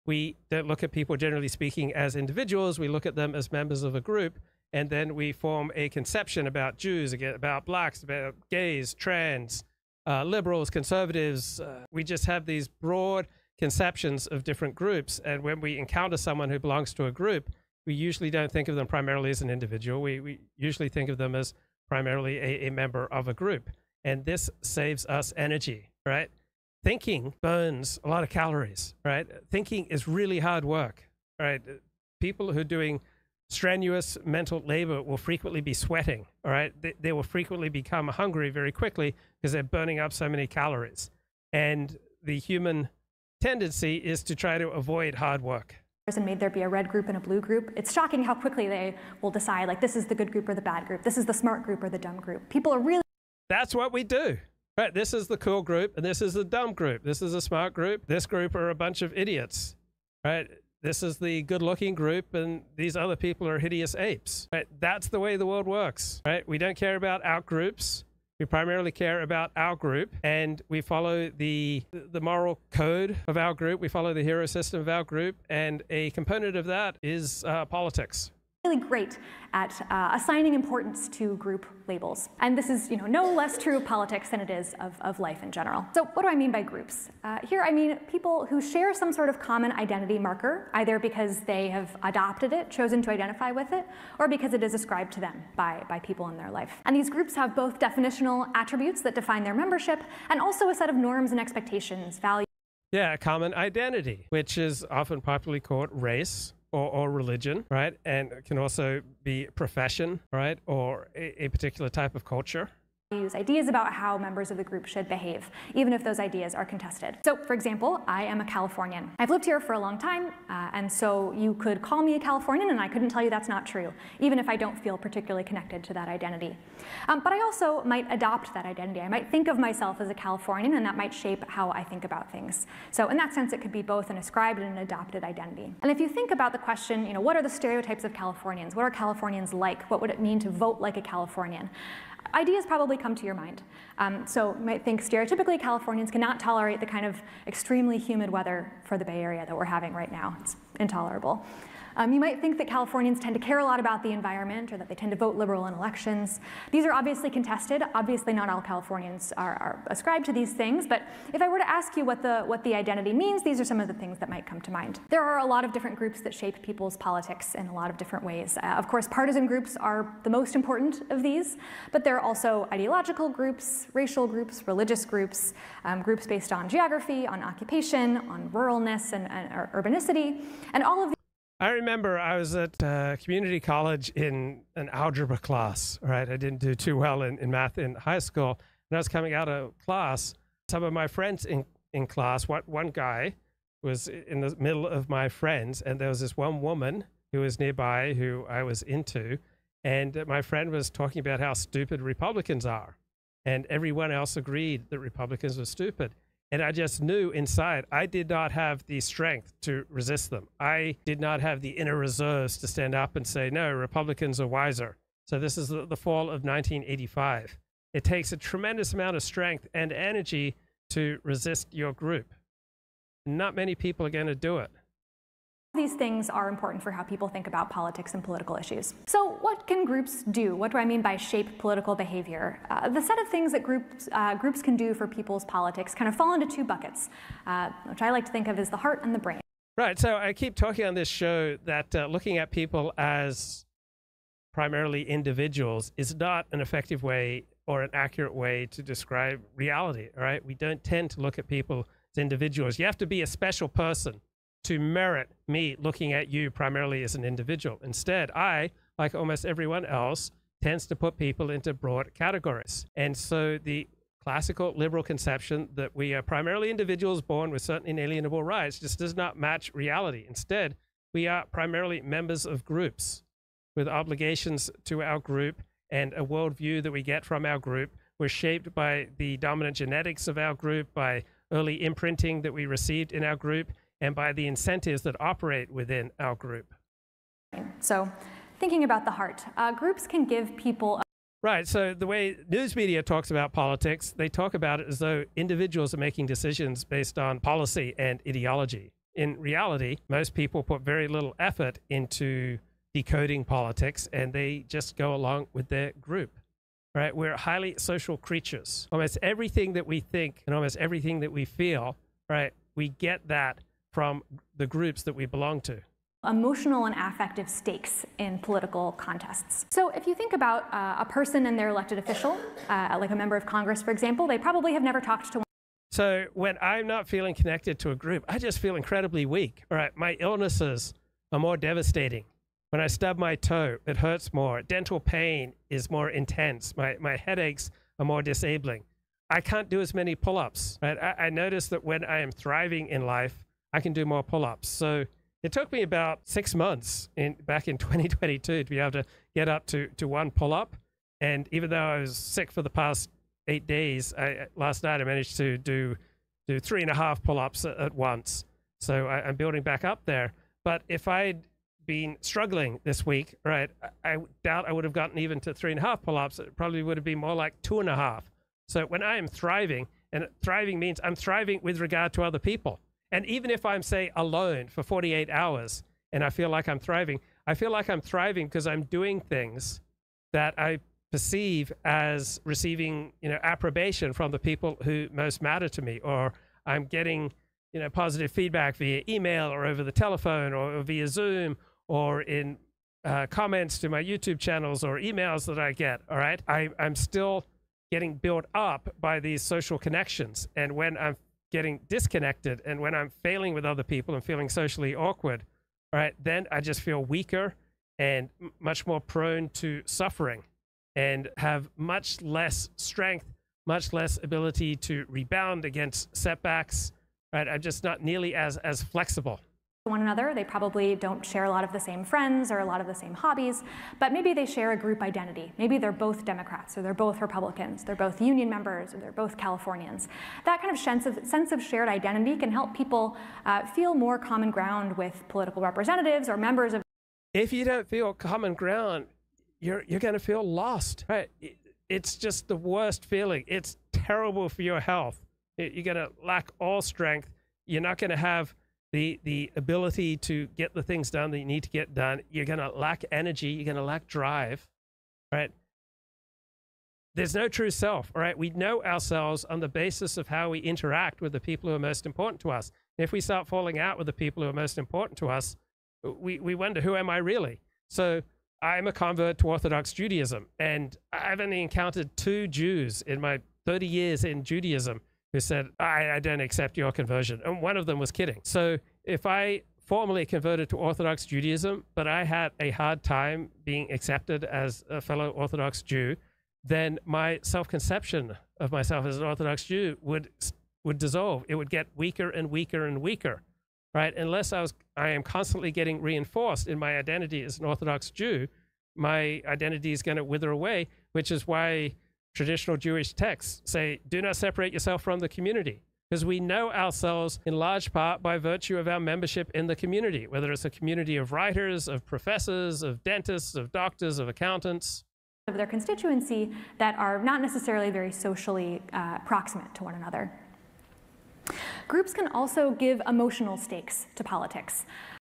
"The Power of Group Identity: How It Shapes our Politics"
Discover how group identity influences how we perceive and interact with others politically. Explore the role of stereotypes, belonging, and emotional stakes in shaping political behavior. Gain insights into the impact of group dynamics on our worldview.